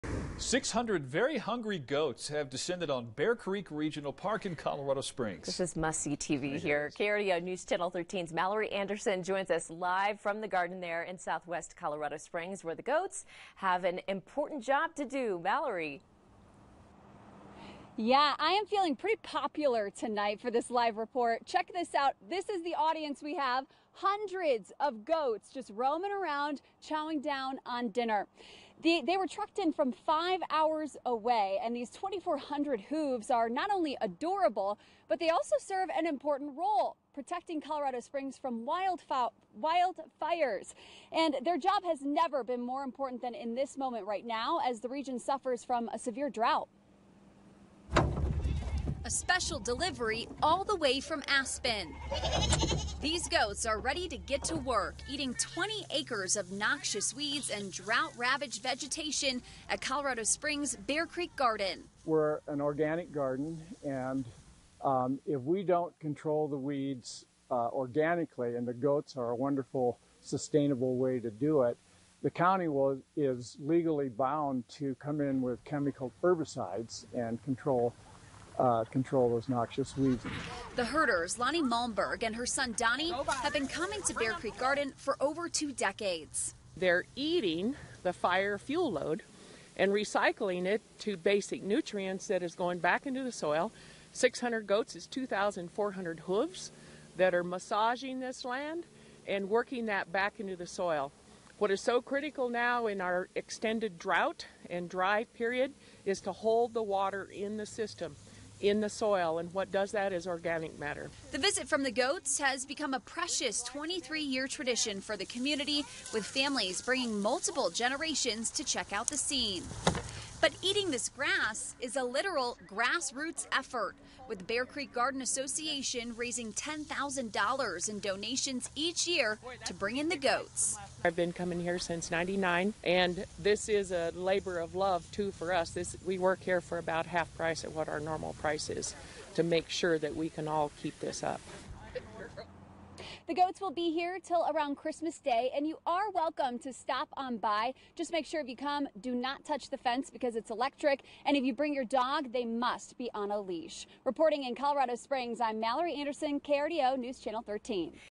600 goats visit Bear Creek Park to help with weeds and fire mitigation
600 goats visit Bear Creek Park to help with weeds and fire mitigation